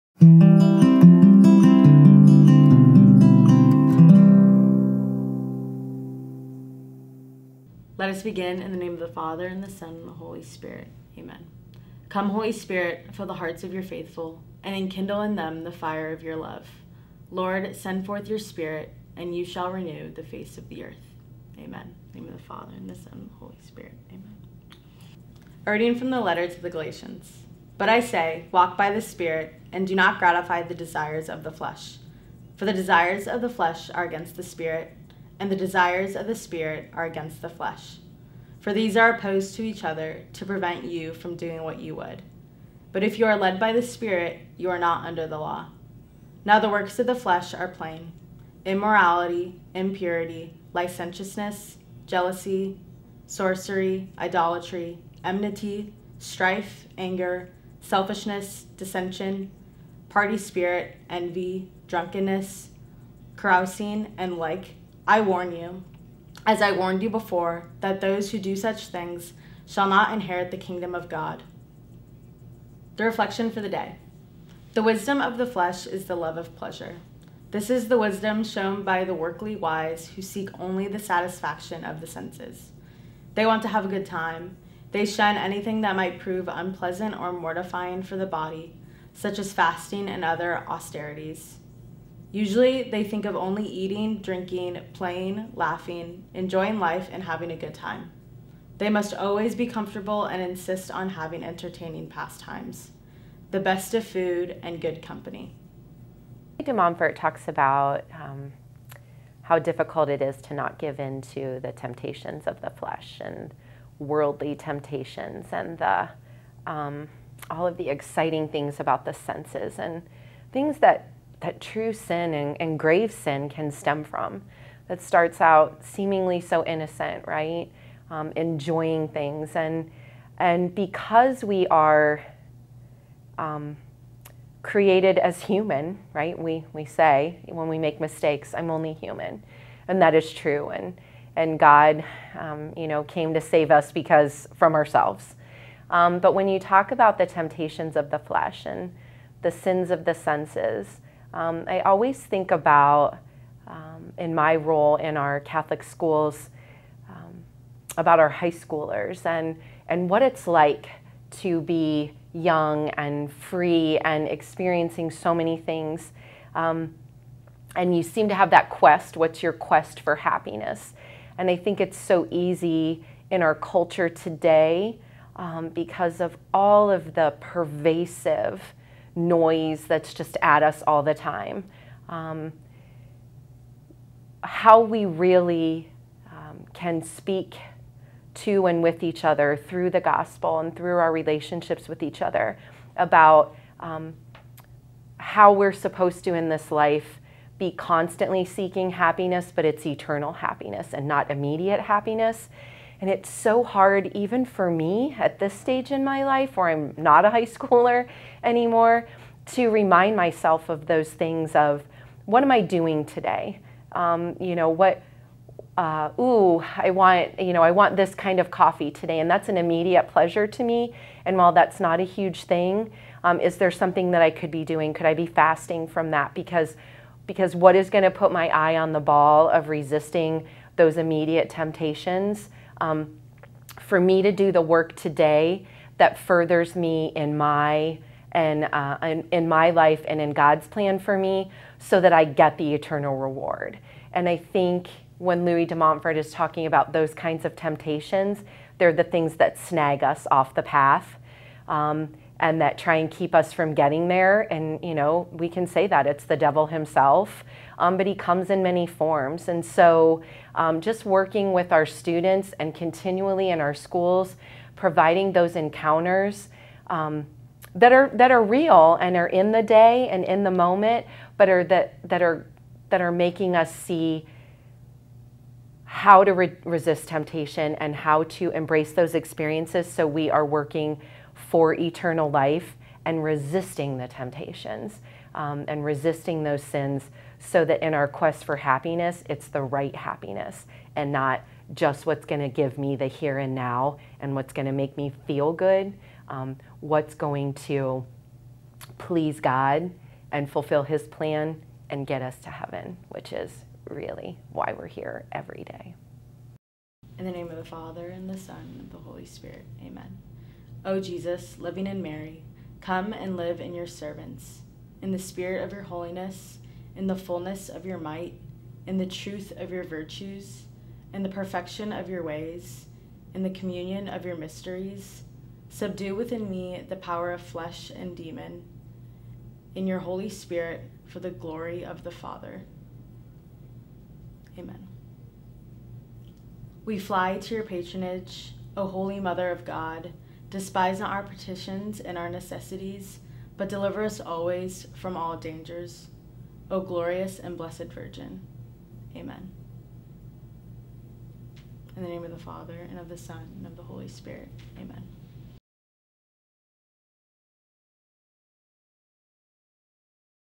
Day 7: The Flesh Is Opposed To The Spirit | Marian Consecration
Day 7 of the Preparation for Total Consecration to Jesus Christ Through Mary closes the first week of preparation by calling to mind the idea that pleasures of the flesh are not in harmony with those of the Spirit. As we continue in this Consecration, we must remember to deny pleasures here on Earth in favor of those that exist in our eternal reward!

Each day of the preparation will be filled with prayers, reflections, and thought-provoking questions meant to prepare our souls for Total Consecration to Jesus Through Mary. For more information on Total Consecration, check out the book we'll be using as our guide:

Follow along and pray with us each day on any of the Diocese of Columbus social media platforms!